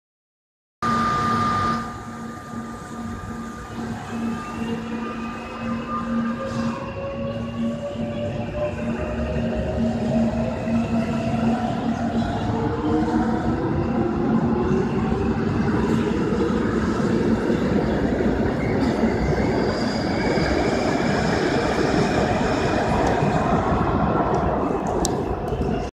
KORAIL 3호선 (韓国鉄道公社3号線3000系) 오금행 수서역 출발영상
372편성 오금행

서울지하철 3호선 수서역

Seoul Subway Line 3 Suseo Stn